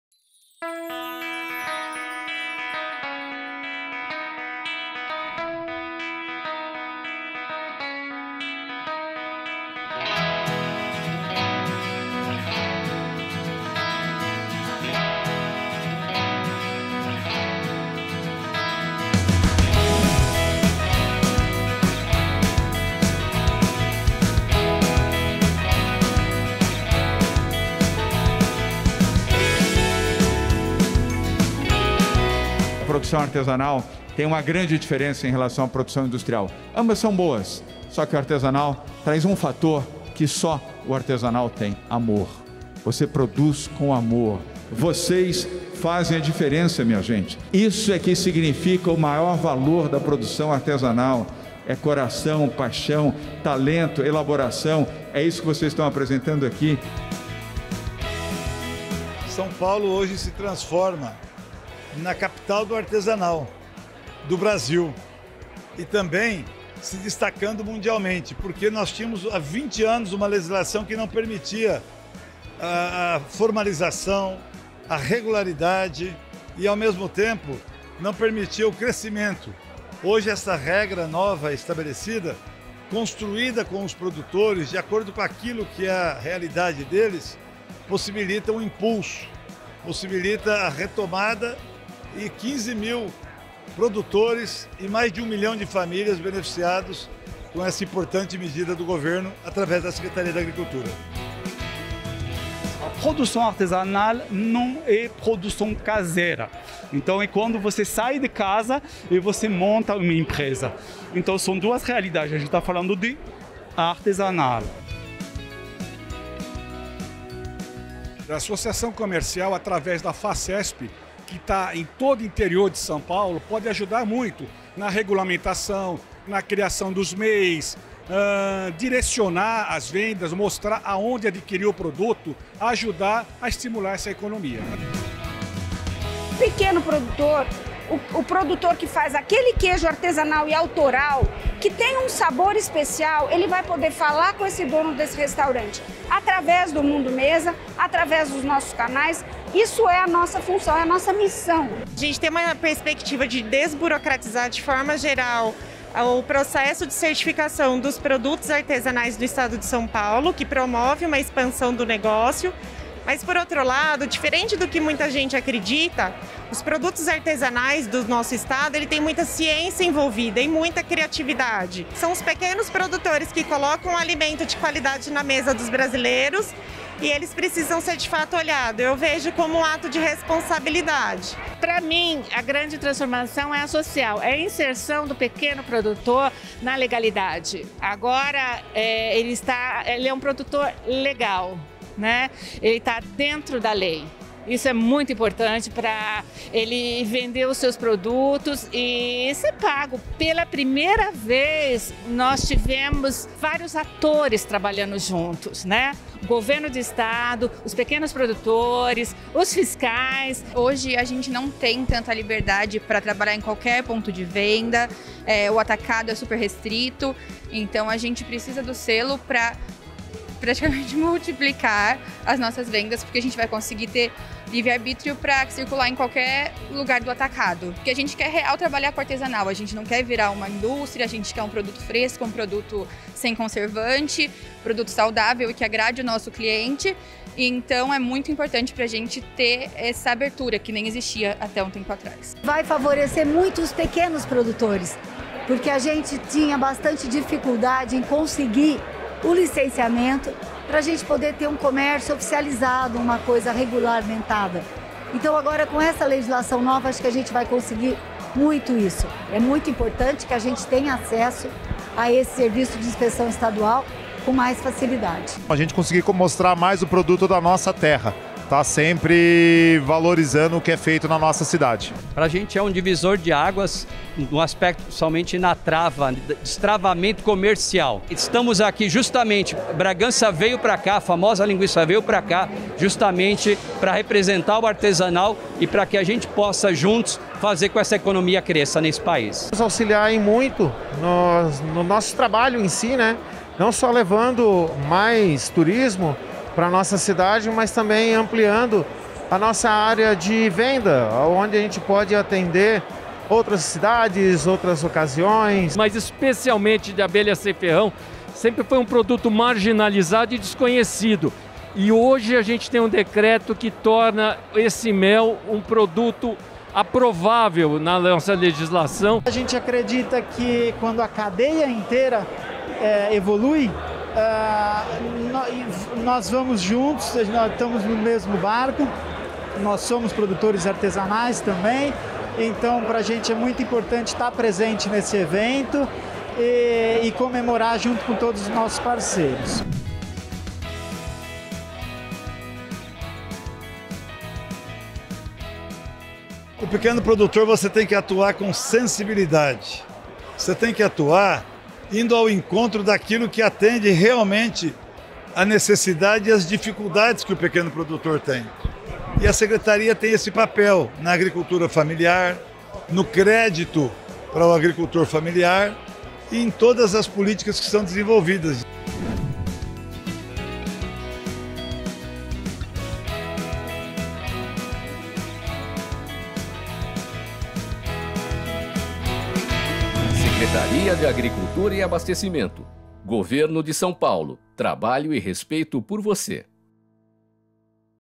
Assinatura do decreto que regulamenta os produtos artesanais de São Paulo
AVANÇO PARA OS ARTESANAIS: Decreto que regulamenta produtos artesanais de origem animal no Estado de São Paulo é assinado!!!

Maior capacidade de desenvolvimento e desburocratização para os produtores artesanais. Mais segurança e opções de produtos de qualidade aos consumidores. Estes são os benefícios com a assinatura do Decreto que regulamenta a lei nº 17.453, de 18 de novembro de 2021, que dispõe sobre a manipulação e o beneficiamento de produtos de origem animal, sob a forma artesanal, bem como sobre sua inspeção e fiscalização no Estado de São Paulo. Assim, o Governo de São Paulo, pela Secretaria de Agricultura e Abastecimento, atende a demanda dos produtores artesanais do Estado

O Decreto foi assinado nesta quarta-feira, 23, pelo Governador João Doria, em ato com o Vice-Governador, Rodrigo Garcia, o Secretário de Agricultura e Abastecimento de São Paulo, Itamar Borges e o Secretário Executivo da SAA, Francisco Matturro.

A formalização permite a comercialização de produtos reconhecidos em todo o mundo, beneficiando 15 mil produtores e cerca de 1 milhão de famílias.

Durante o evento também foi lançado o selo “Artesanal de São Paulo”, que já está disponível para download para uso dos produtores, gratuitamente, no site da Secretaria de Agricultura e Abastecimento.